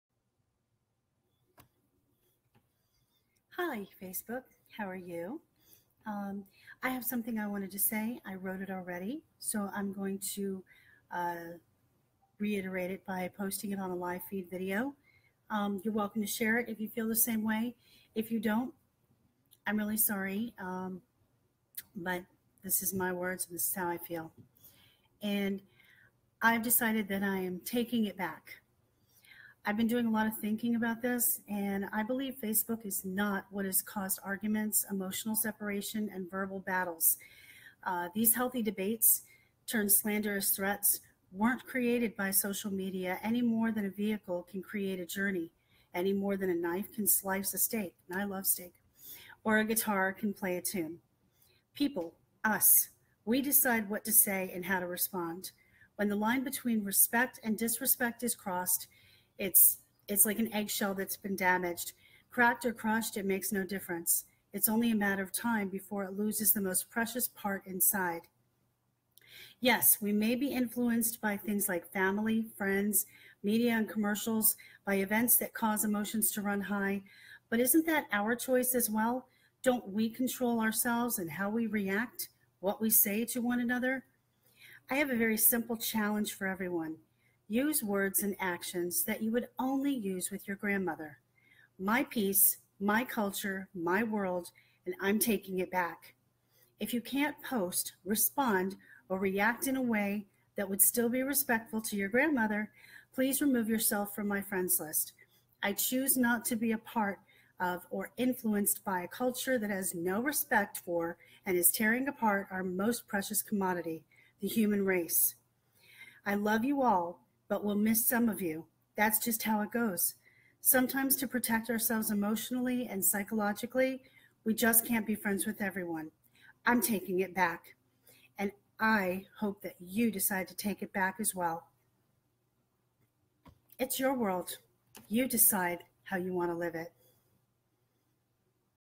Taking It Back- I am taking back control of what I see and hear on social media.
I have been doing a lot of thinking about this. I believe Facebook is NOT what has caused arguments, emotional separation and verbal battles. These healthy debates turned slanderous threats weren't created by social media any more than a vehicle can create a journey. Any more than a knife can slice a steak (I love steak, lol!) or a or a guitar can play a tune.
People. Us. We decide what to say & how to respond. When the line between respect and disrespect is crossed it's like an eggshell that has been damaged. Cracked or crushed, it makes no difference. It's only a matter of time before it loses the most precious part inside.
Yes, we may be influenced by things like family, friends, media and commercials, by events that cause emotions to run high, but isn't THAT our choice as well? Don't WE control ourselves and how we react; what we say to one another?
I have a very simple challenge for everyone. Use words and actions that you would only use with your grandmother.
My peace, my culture, my world. I am
If you can't post / respond/ react in a way that would still be respectful to your grandmother, please remove yourself from my friends list. I choose NOT to be a part of or influenced by a culture that has no respect for and is tearing apart our most precious commodity, the human race.
I love you all but will miss some of you, that's just how it goes. Sometimes to protect ourselves emotionally and psychologically we just can't can't be friends with everyone. Respect and kindness: I'm TakingItBack, and I hope you do too. -Siren Says